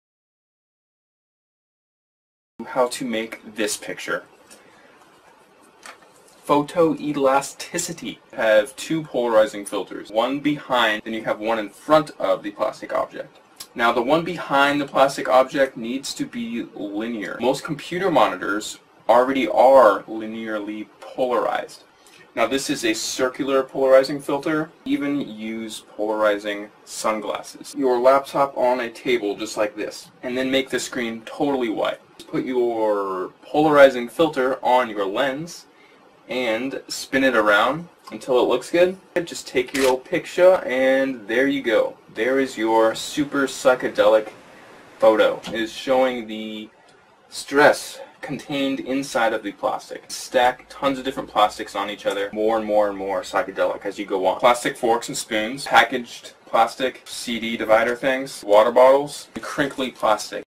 Photoelasticity
Use polarization to photograph psychedelic stress patterns in hard plastic objects.